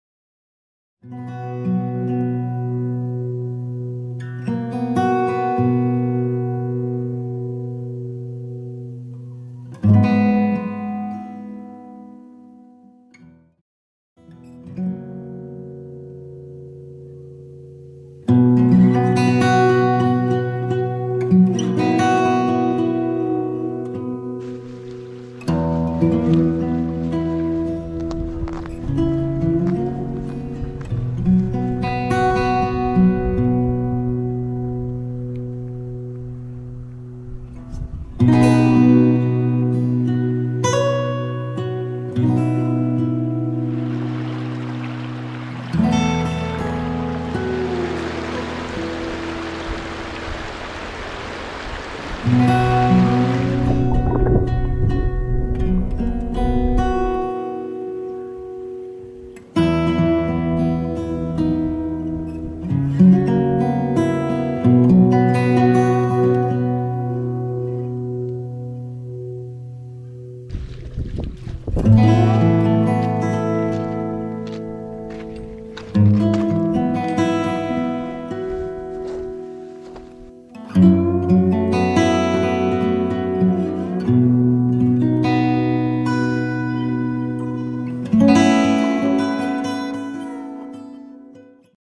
Fly Fishing the Conejos Featurette
This video is a preview of the 10 days I spent fly fishing on Colorado's Conejos River in August.